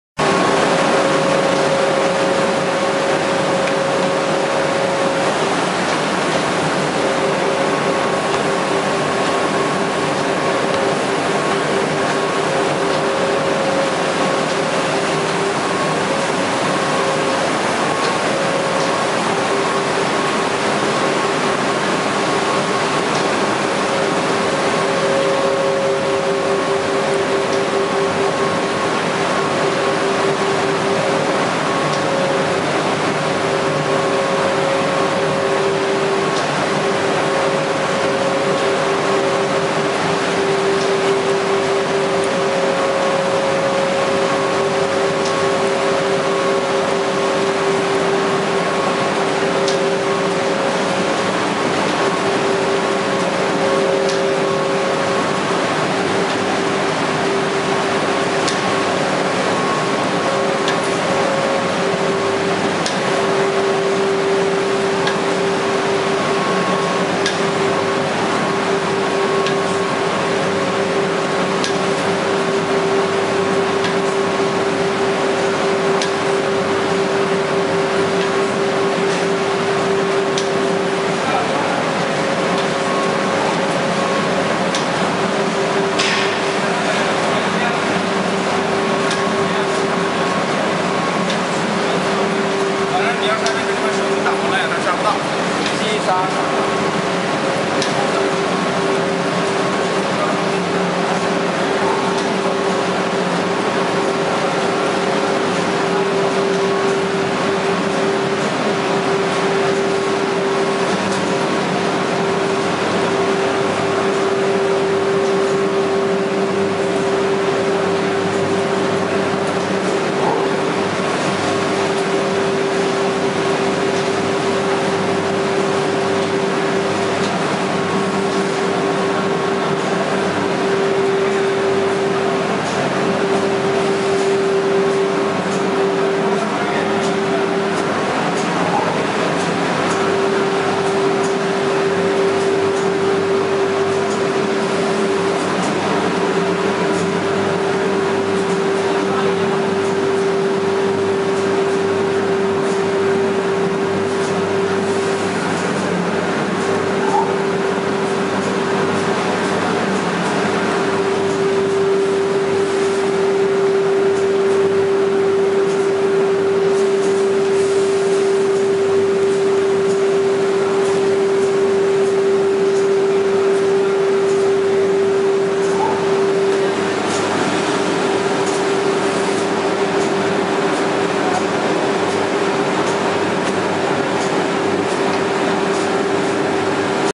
UV coating line/MDF UV painting production line
Designed to paint solid wood, MDF, laminated panels, etc all flat surface work piece.